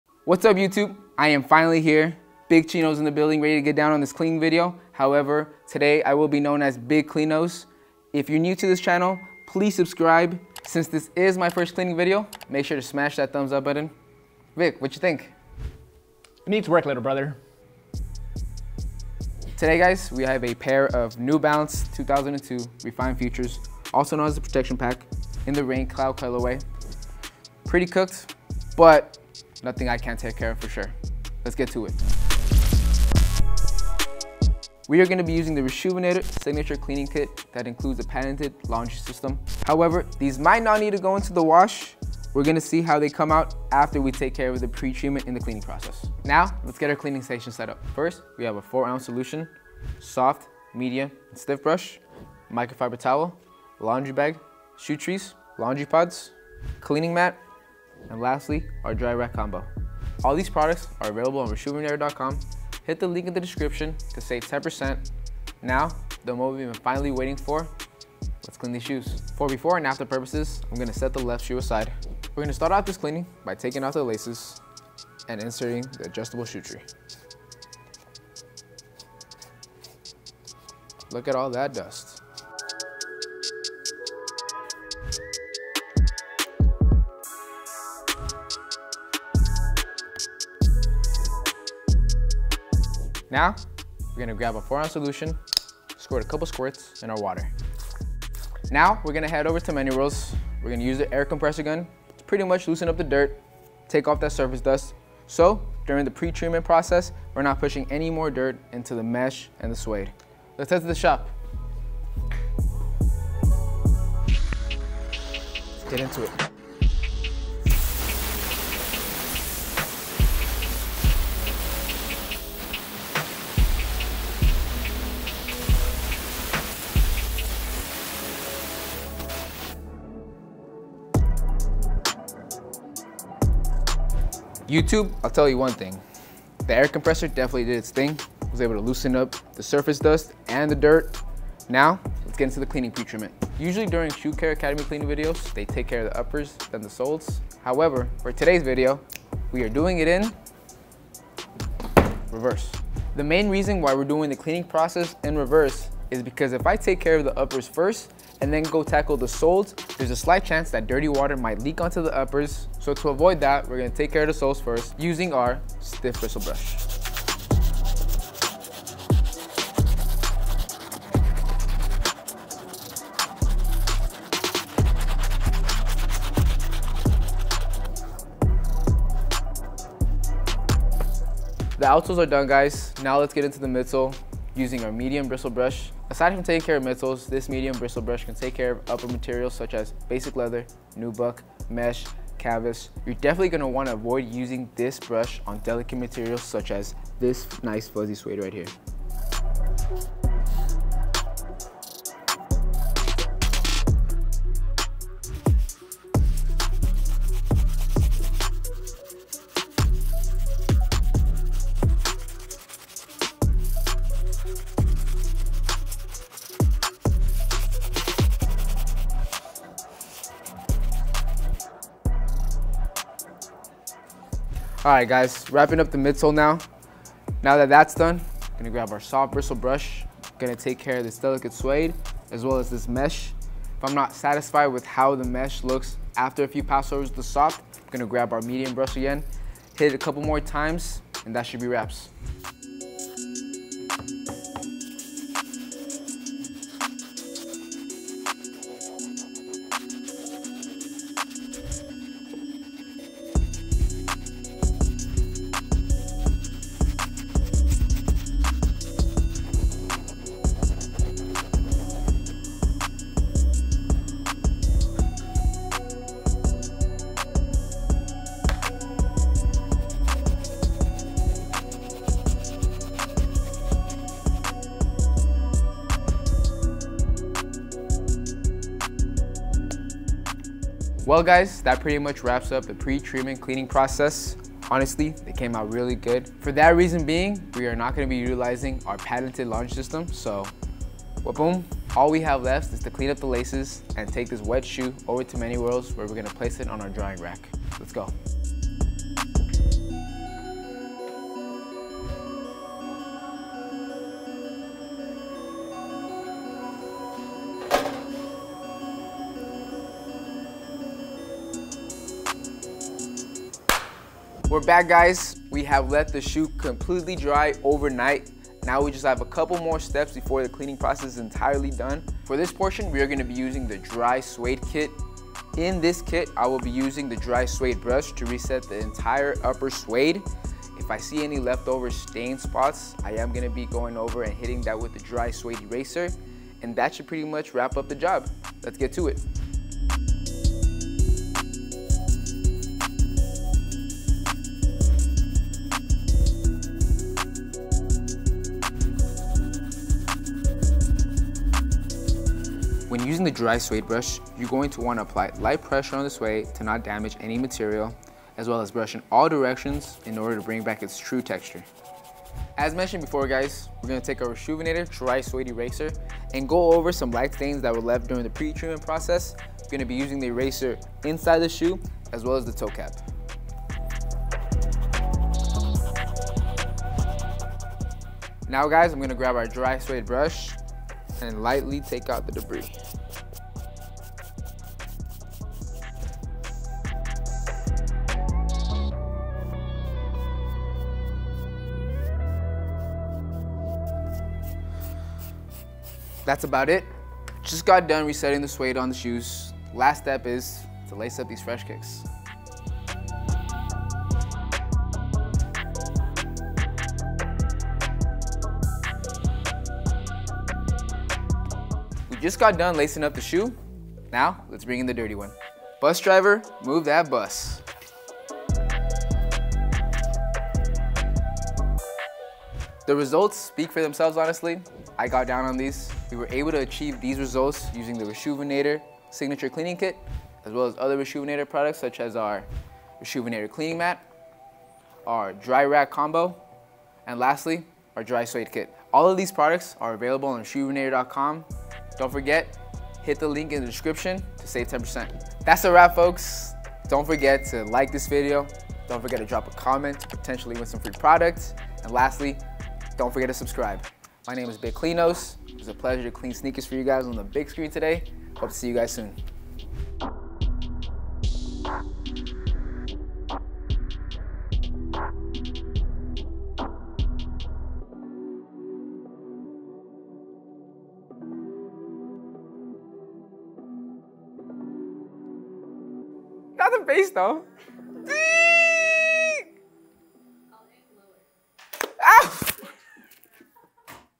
How To Clean Suede & Mesh Shoes | New Balance 2002R
This might not be Vick, but he did a number on these New Balance 2002R sneakers! In today's Shoe Care Academy video Yared will walk you through a step by step tutorial on how to clean suede and mesh sneakers. These are known as the Protection Pack to most, but the real name is the Redefined future... Did you know that?!

To clean these up we used the RESHOEVN8R Signature Cleaning Kit and paired it with some of our other favorite products. Looking at these sneakers we thought for sure they would need the washing machine due to the mesh material and the amount of dirt stuck in there, but to our surprise these cleaned up great with just the pretreatment.

The pretreatment consisted of three different sneaker cleaning brushes and an all natural Cleaning Solution. The Stiff Bristle Brush was used to break down all of the grime on the outsole before moving onto the beat uppers. Once the sole was clean it was time to use the Medium Bristle Brush on the midsole. Finally it was time to use the Soft Brush to create all the bubbles and flush out those tricky upper materials. Once the sneaker was clean all that was left to do was let it air dry on our drying rack.

Once the sneaker was dry it was time to reset that nap on the suede material. We used the Suede Brush and applied very minimal pressure to bring the suede back to a buttery texture. Once that was done we used the suede Eraser to remove the trapped in stains that didn't come out from the pretreatment.

Overall these cleaned up great! Fun fact... These were worn by our Global Sales Manager for over a year before we took on this cleaning!

Give Yared some love for his first ever cleaning video by dropping a comment!